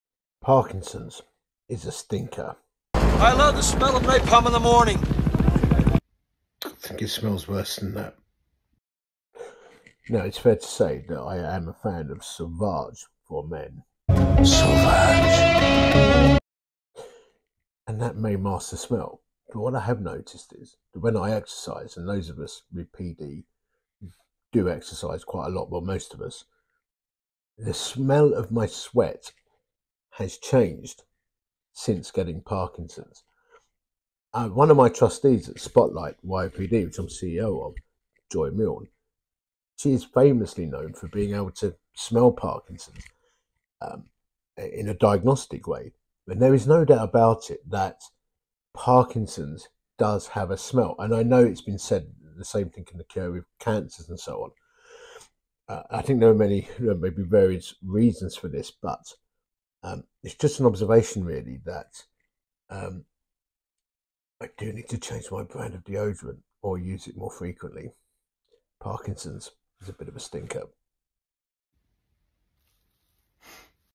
Parkinson’s Stinks (yes it has a smell)
Parkinson’s, like some other diseases actually has a smell.